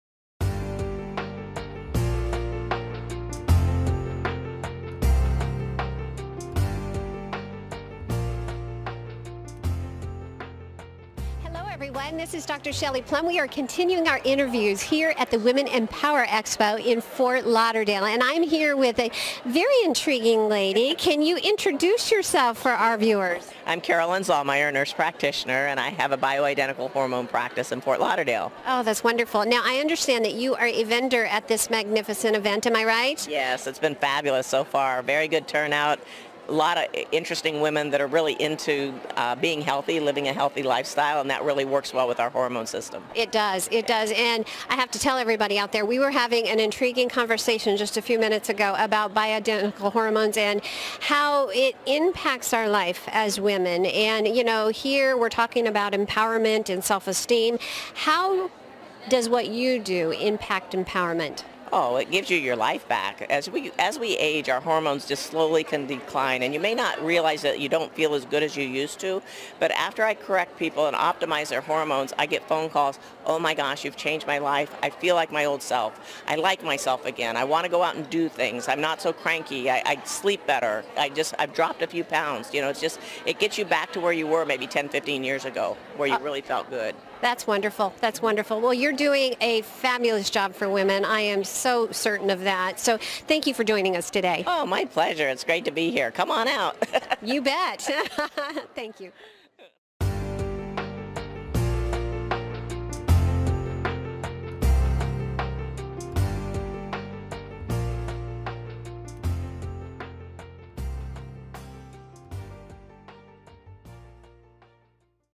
Change Your Life?
An inspiring woman! Meet nurse practitioner Carolyn Zaumeyer as she discusses the concept of bio-identical hormones.  Can they change lives for women worldwide?  Let's find out!